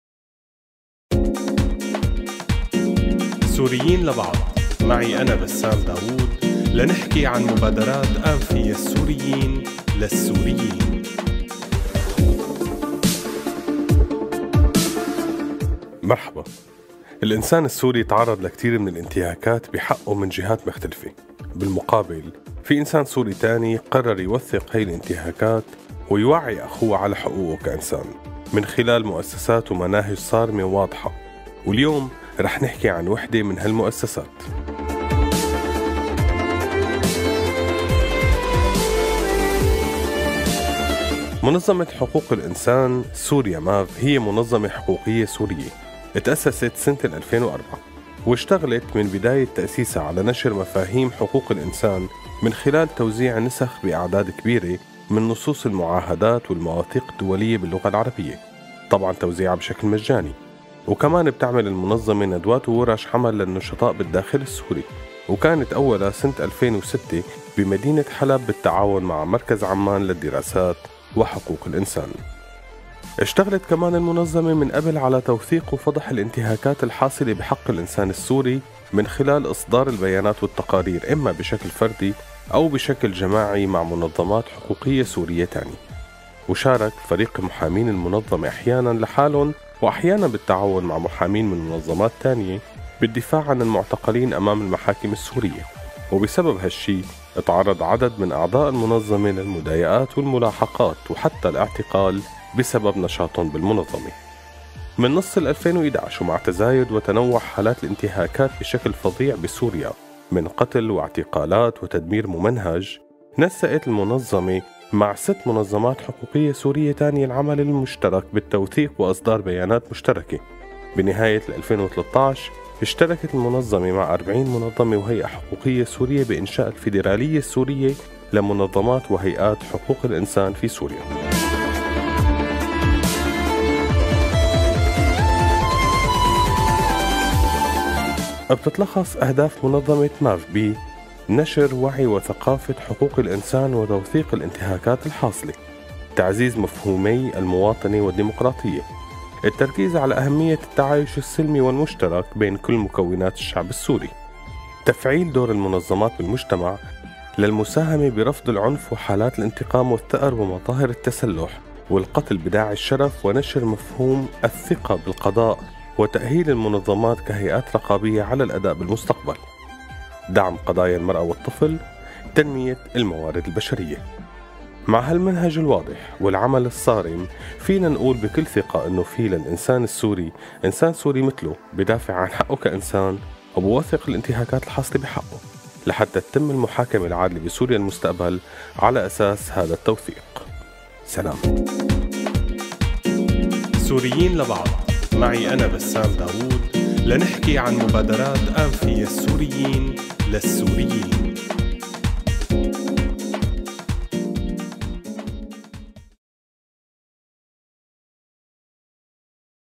سوريين لبعض 76 - منظمة حقوق الإنسان في سوريا - ماف
صحيح حقوق الإنسان السوري منتهكة، بس في مؤسسات ومنظمات عم تشتغل عالتوعية والتوثيق، مشان الكل يخضع للعدالة، ومن هي المنظمات، منظمة حقوق الانسان في سوريا - ماف اللي منعرف عنها أكتر بهالحلقة.